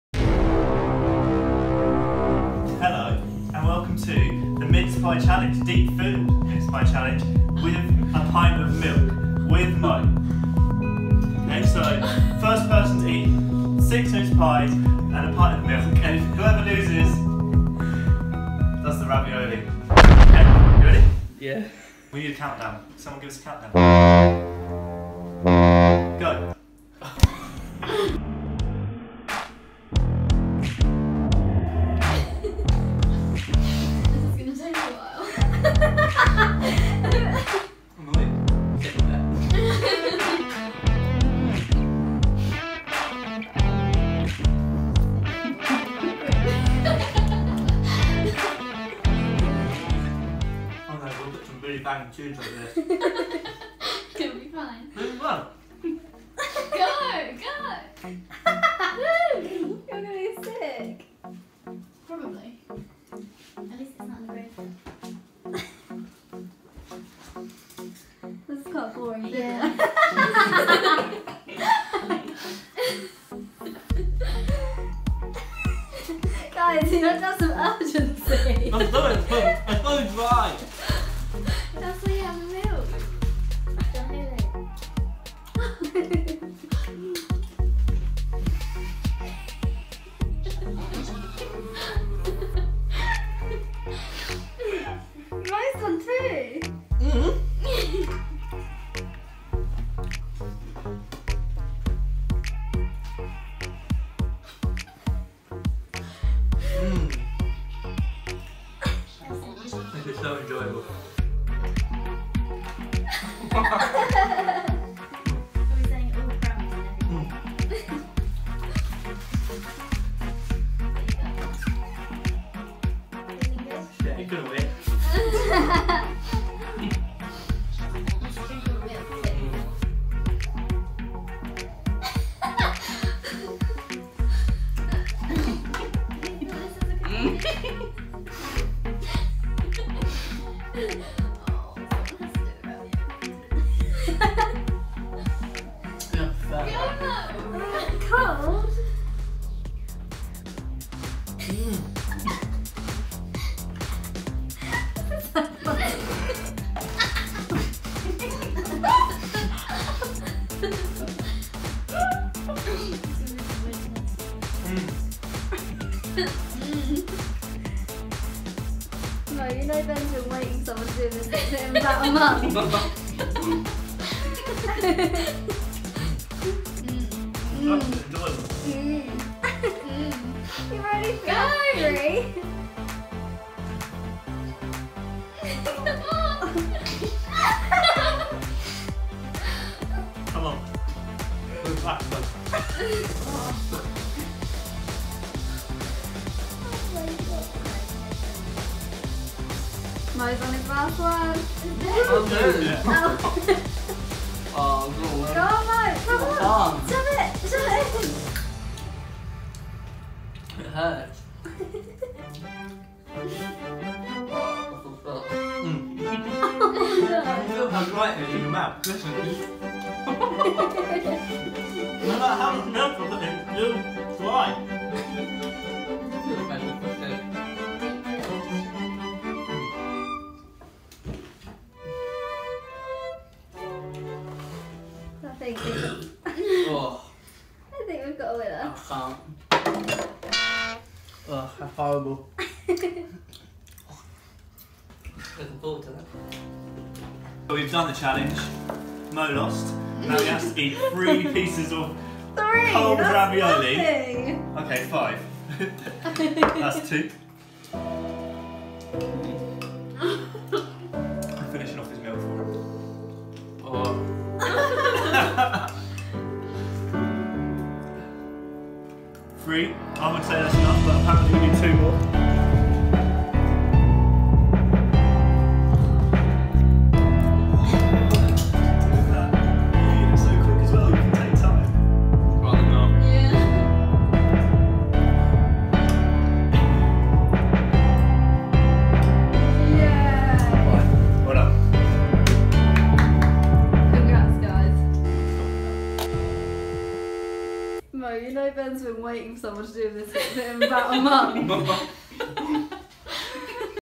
The Six Deep-Filled Mince Pie Challenge
Introducing the first Mohammad vs Benjamin challenge! A Christmas extravaganza for your entertainment🎉🎁🎅🏼